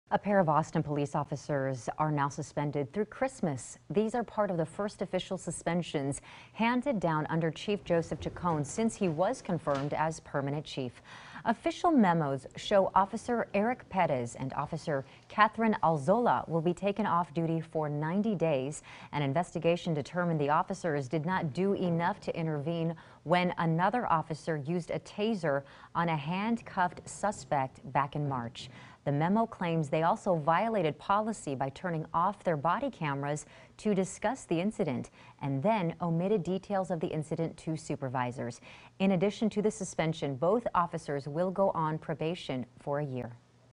Austin officers suspended for not properly reporting details of March use-of-force incident
At least two Austin police officers were suspended for 90 days for failing to step in and properly report details on a March 12 incident involving force, according to city memos from Austin Police Chief Joseph Chacon.